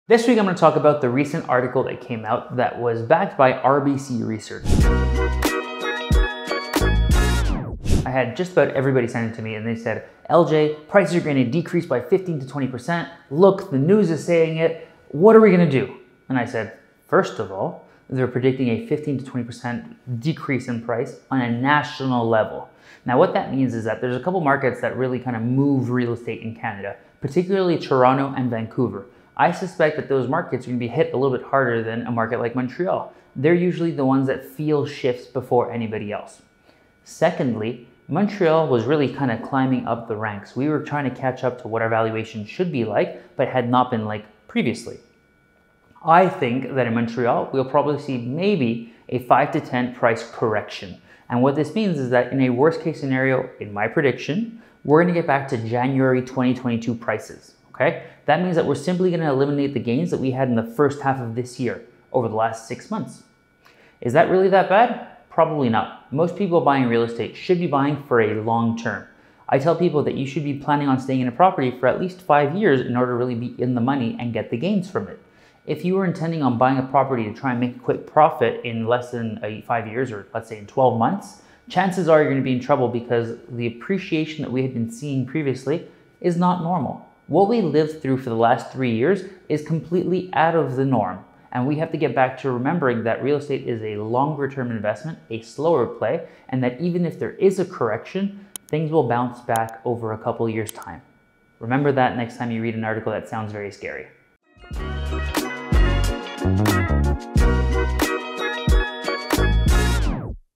My Thoughts on the Forecast for Canada's Housing Market
This week I’ll be discussing an article that was backed by RBC Research about a forecast on the Canadian housing market. It has been sending people into a bit of tailspin because it predicted a price decrease of 15%-20%. Now, I do not believe there is any reason to panic because this prediction is on a national level. The markets that really move real estate in Canada are the major ones, such as Vancouver and Toronto, and these are the markets that will feel a shift before Montreal does. We may see a correction of 5%-10% here, which may bring us back to January 2022 prices. When someone buys a property, I tell them to plan to stay there for at lease 5 years to really be in the money. So even if there is a correction, things will bounce back over a couple of years and your investment will be fine. What are your thoughts on this forecast?

LJ Aguinaga
CERTIFIED REAL ESTATE BROKER | LJ REALTIES